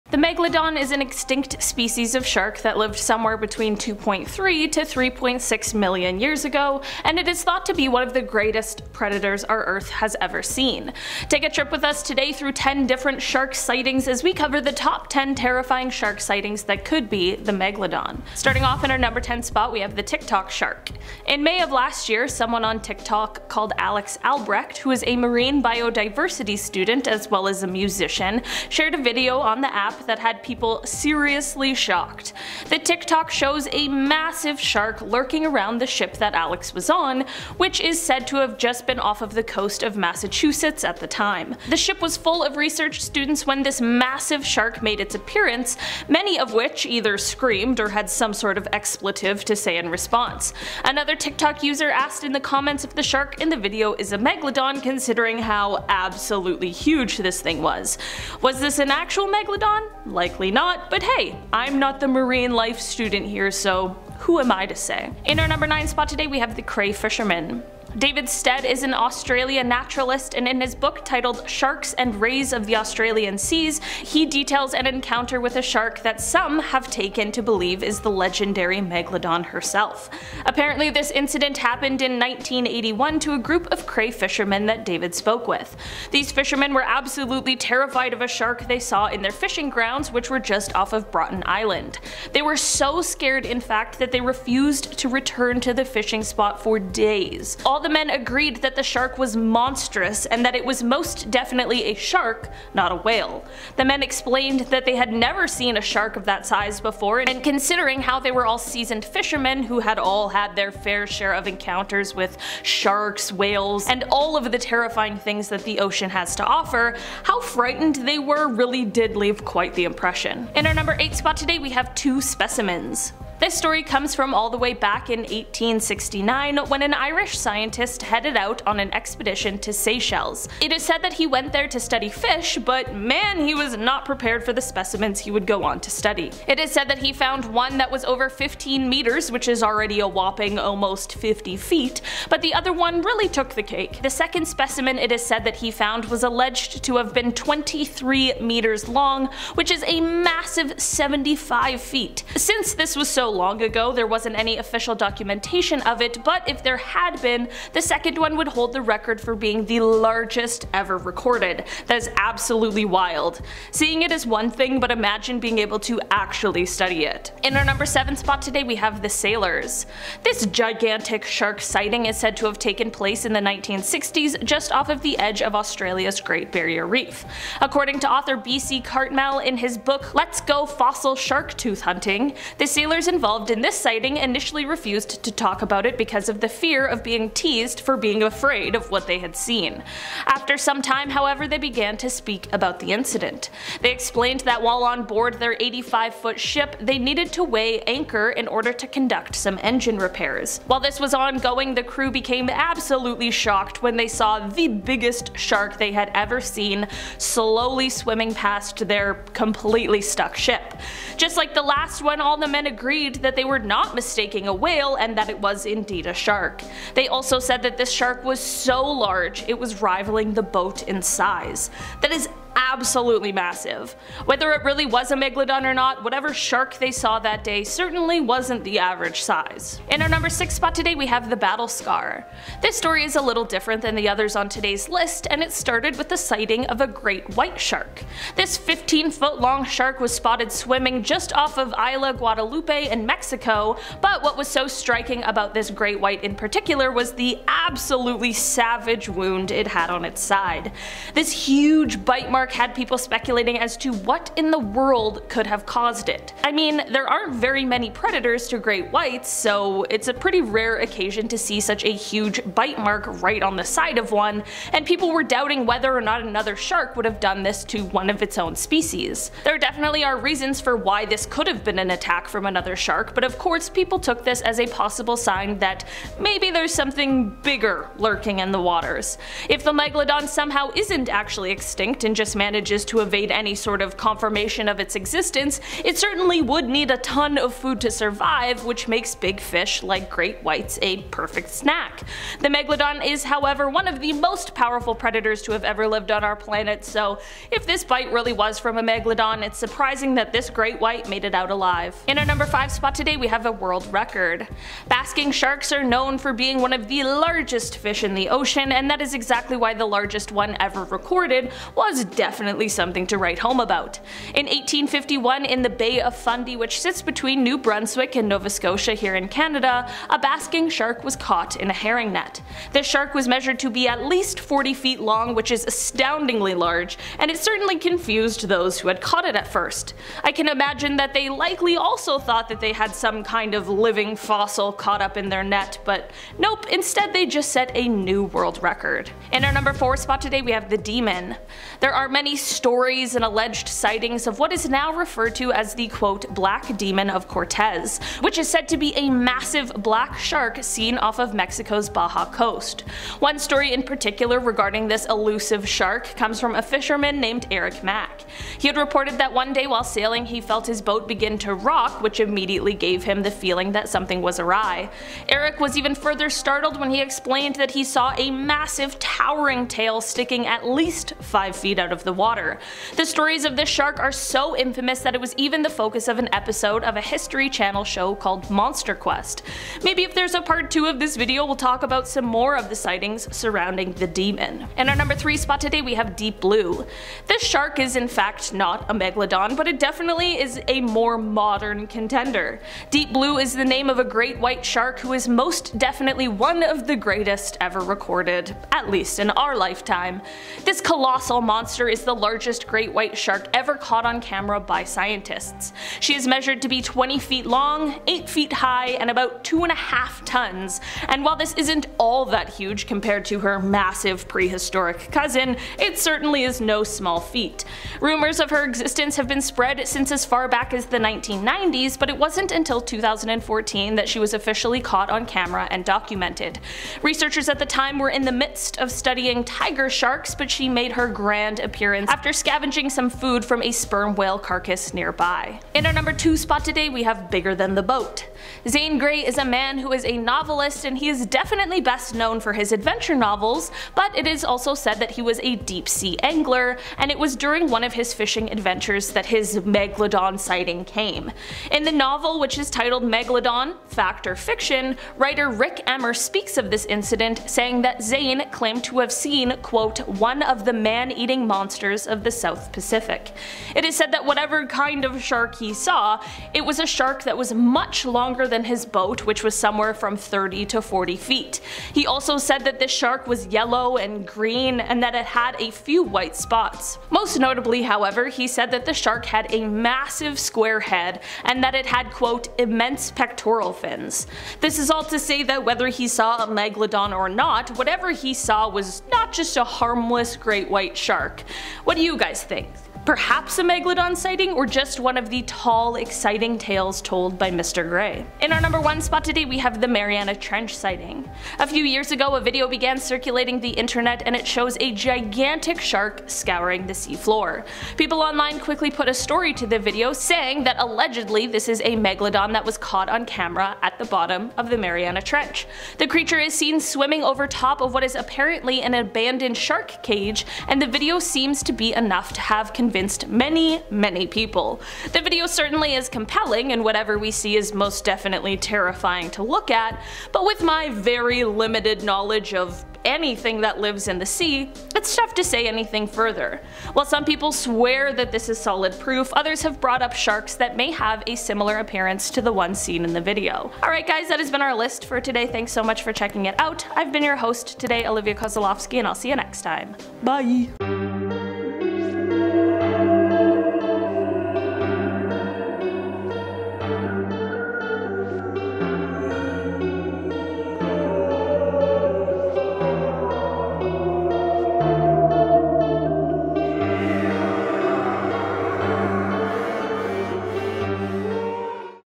Top 10 Terrifying Shark Sightings That Could Be The Megalodon
The Megalodon is an extinct species of shark that lived somewhere between 2.3 to 3.6 million years ago and is thought to be one of the greatest predators our Earth has ever seen. Take a trip with us today through 10 different shark sightings as we talk about the Top 10 Terrifying Shark Sightings That Could Be The Megalodon.

Time Codes:
0:00 Intro
0:18 Tik Tok Shark
1:05 The Cray Fishermen
2:00 Two Specimens
2:47 The Sailors
3:50 The Battle Scar
5:02 World Record
5:39 The Demon
6:25 Deep Blue
7:15 Bigger Than The Boat
8:18 The Mariana Trench Sighting